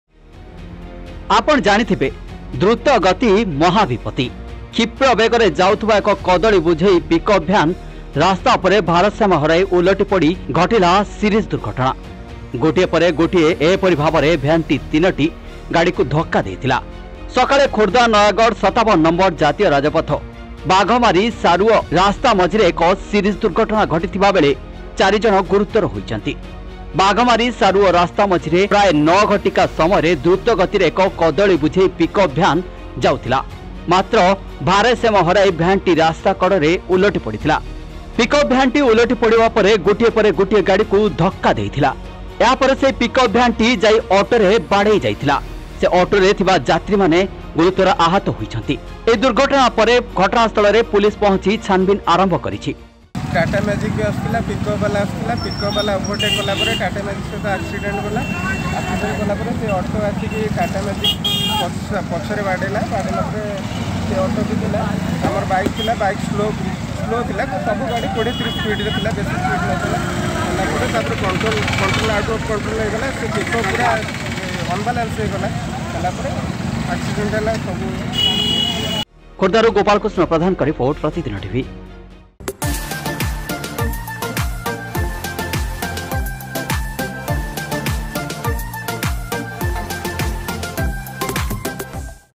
ଓଲଟିଲା କଦଳୀ ବୋଝେଇ ପିକ୍ଅପ ଭ୍ୟାନ, ୫ଆହତ | PratidinTv
ଓଲଟିଲା କଦଳୀ ବୋଝେଇ ପିକ୍ଅପ ଭ୍ୟାନ, ୫ଆହତ